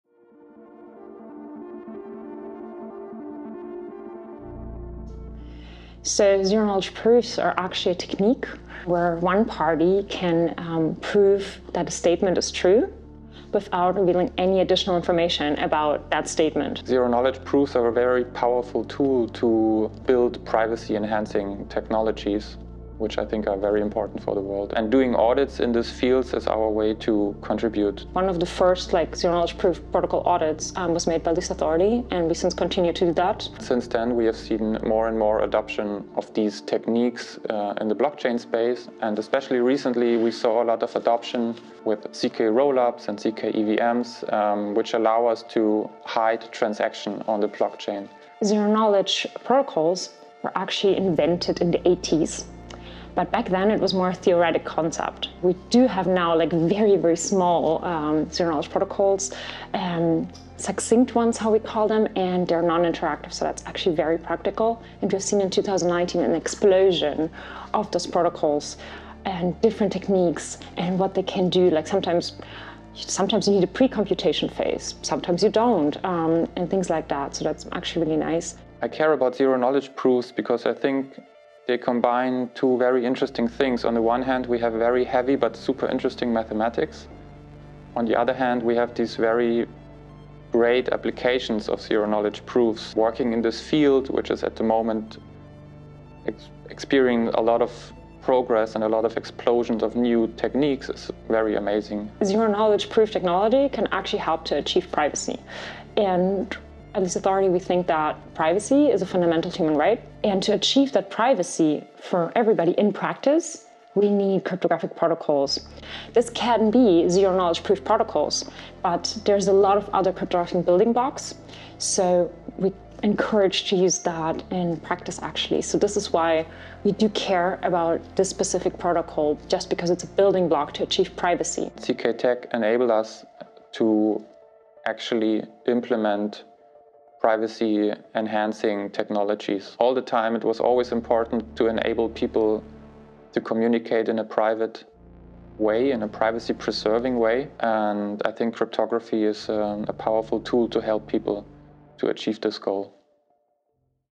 Zero Knowledge Technology
Cryptography provides the building blocks for Zero Knowledge protocols and technology which is a powerful tool to build and implement privacy enhancing technologies.